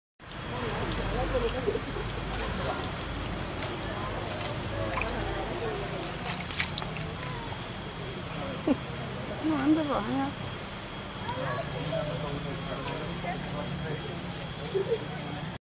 apalekerivattnet.avi
Sacred Monkey Forest, Ubud, Bali. A curious monkey plays in the water.